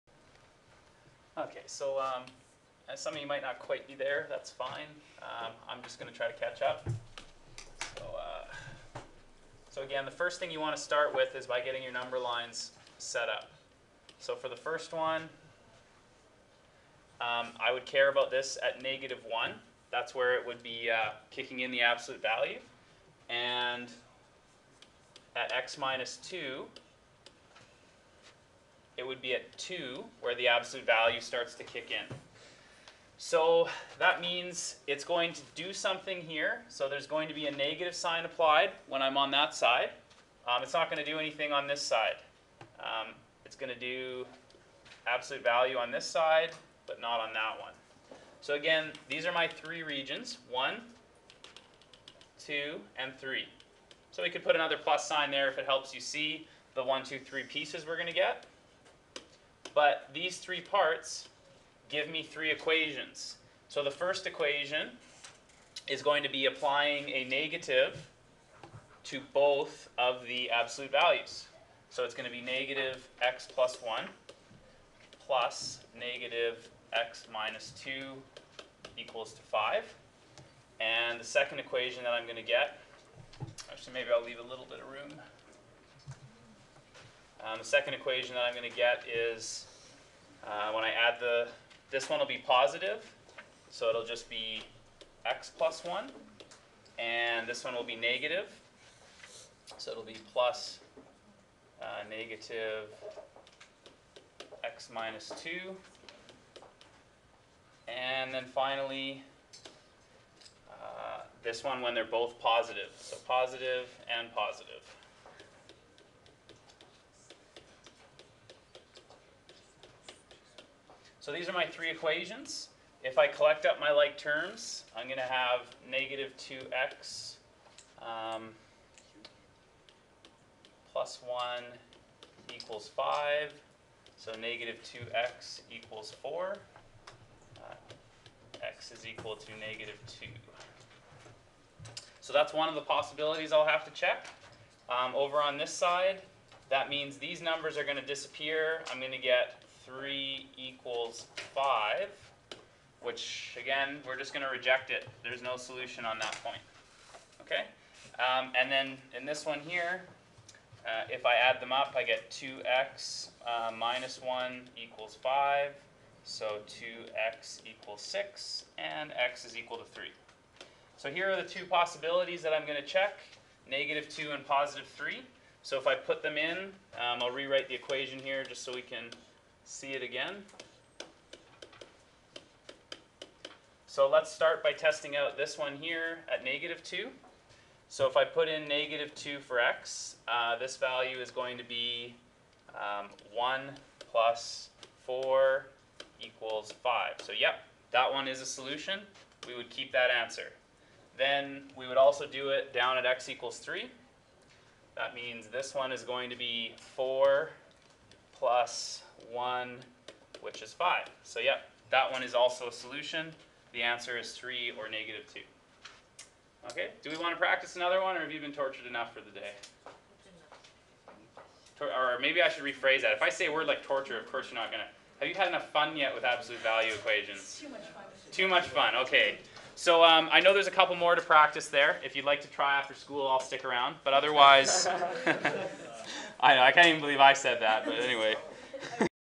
Mr Joyce Math 11 - Solving Absolute Value Equations IV
We looked at the absolute value function and solving equations that use it in this lesson